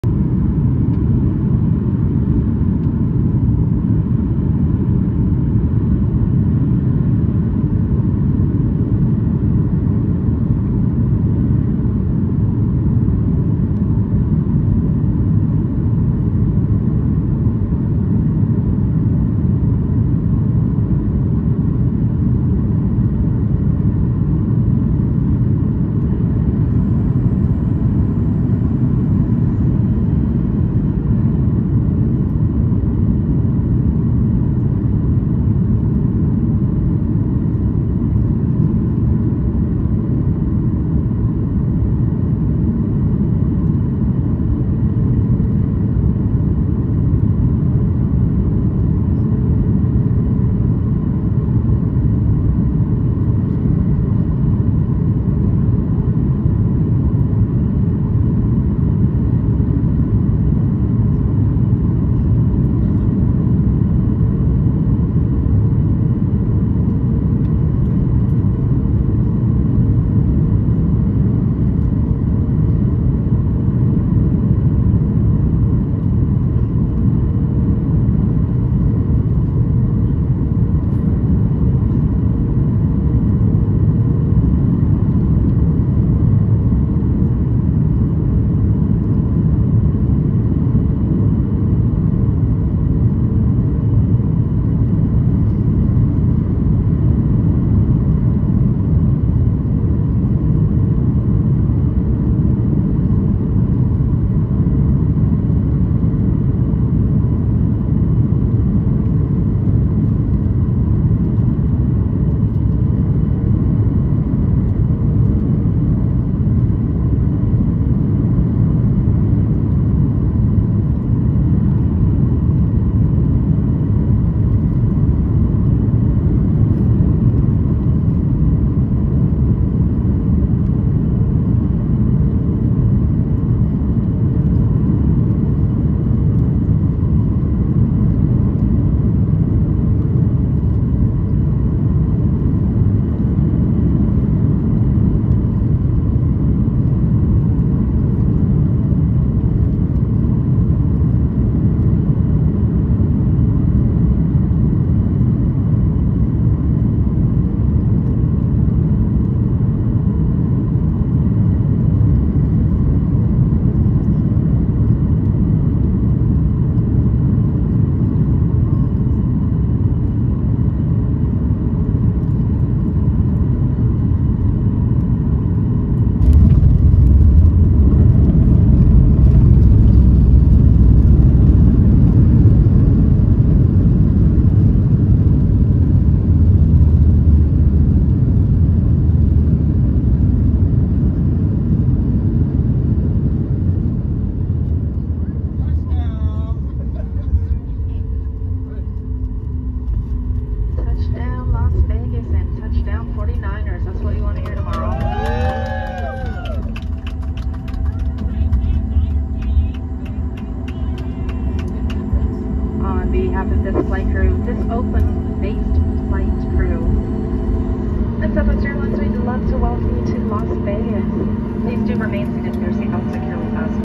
Touchdown! - Southwest Airlines MAX 8 Landing at Harry Reid International Airport
Touchdowns were ever evident on this flight and part of the theme of this flight as this Oakland based crew and pax were very excited for the Super Bowl matchup taking place in Vegas the next day! Full of 49er fans, this flight was an energetic one considering we landed a little after 7:30am. Unfortunately, the Niners did end up losing the Super Bowl so I'd be curious to know what the mood was like coming home.

Flight Information:
Departure Airport: KOAK
Arrival Airport: KLAS
Runways Used: 1L
Flight Time: 0:58
Flight #: WN185